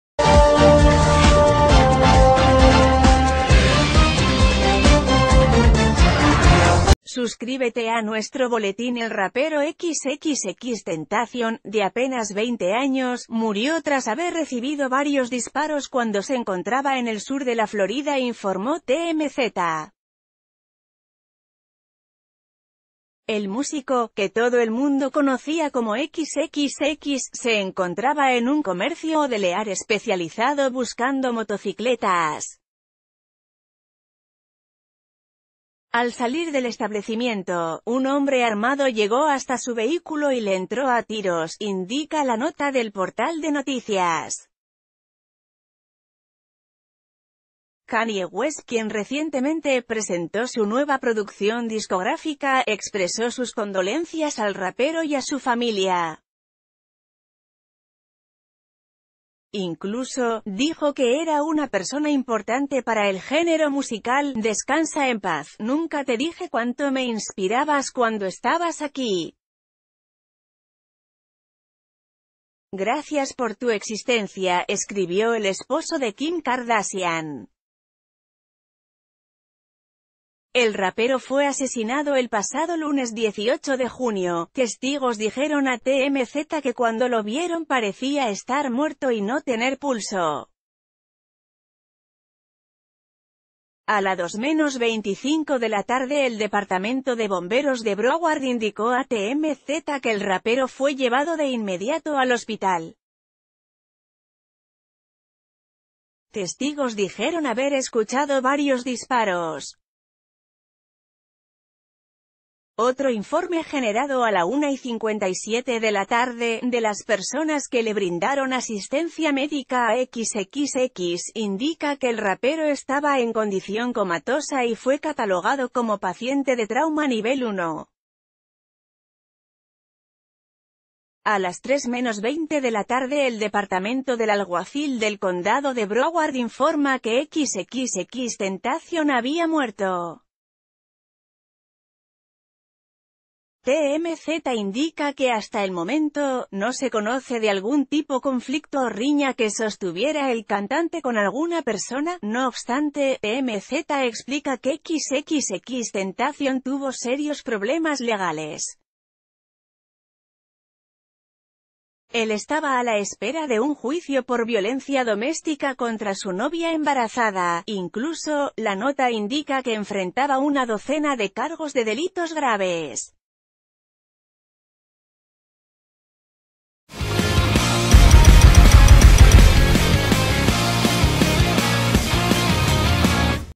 El rapero XXXTentacion muere a balazos a los 20 años
El músico, que todo el mundo conocía como XXX, se encontraba en un comercio o delear especializado buscando motocicletas. Al sal...